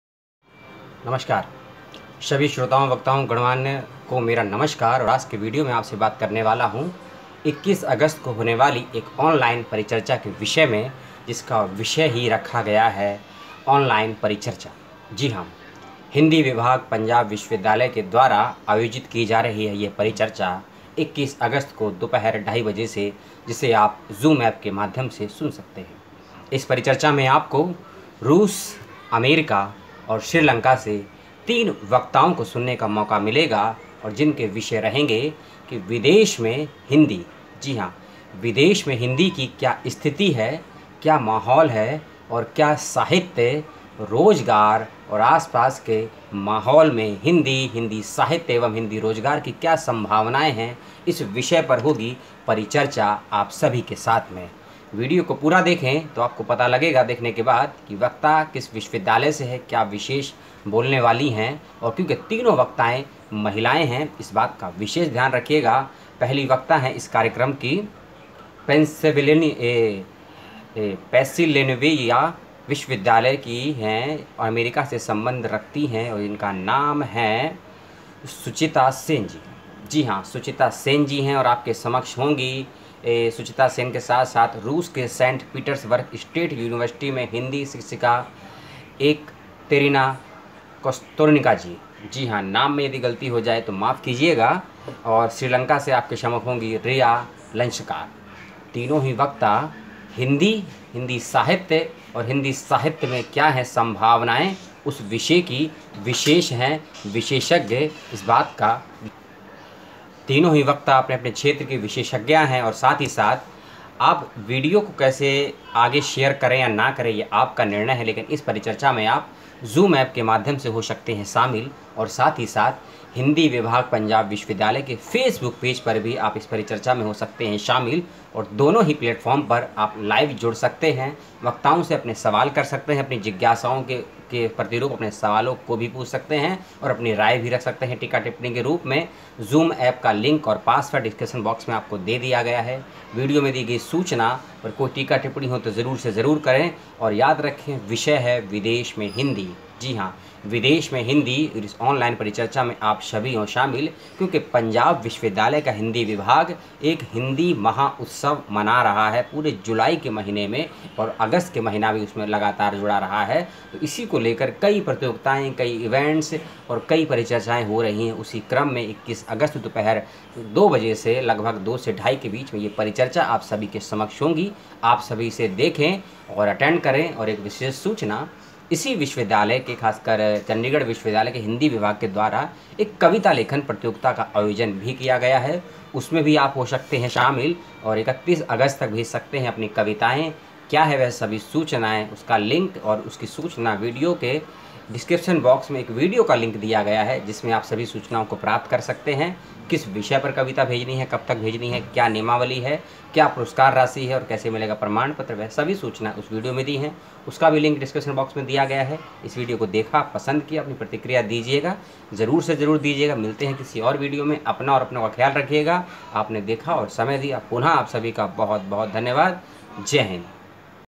विदेश में हिंदी @वेबिनार शुचिस्मिता सेन एकातेरीना कोस्तिना रिद्मा लंसकार @पंजाब विश्वविद्यालय
हिंदी विभाग द्वारा विदेश में हिंदी पर वेब - संवाद

पंजाब विश्वविद्यालय के हिंदी विभाग द्वारा हिंदी माह उत्सव के तहत आयोजित विशेष व्याख्यान/परिचर्चा श्रृंखला की दूसरी कड़ी में शुक्रवार दिनांक 21 अगस्त 2020,2:30 pm को परिचर्चा का आयोजन किया जाएगा।

इस कार्यक्रम में विशेष रूप से अमेरिका के पेंसिलवेनिया विश्वविद्यालय से हिंदी पाठ्यक्रम की प्रमुख शुचिस्मिता सेन, रूस के सेंट पीटर्सबर्ग स्टेट यूनिवर्सिटी में हिंदी शिक्षिका एकातेरीना कोस्तिना और श्रीलंका के राष्ट्रीय भाषा शिक्षण व प्रशिक्षण संस्थान से रिद्मा लंसकार मुख्य वक्ता होंगी।

इस वेब - संवाद में हिस्सा लेने वाले प्रतिभागियों को विदेश में हिंदी भाषा की स्थिति के बारे में जानने का अवसर मिलेगा। कार्यक्रम में आप नीचे दिए गए लिंक के माध्यम से जुड़ सकते हैं।

PASSWORD:-hindi
हिंदी हैं हम कविता लेखन प्रतियोगिता @Poetry Writing Competition @हिन्दी माह उत्सव 2020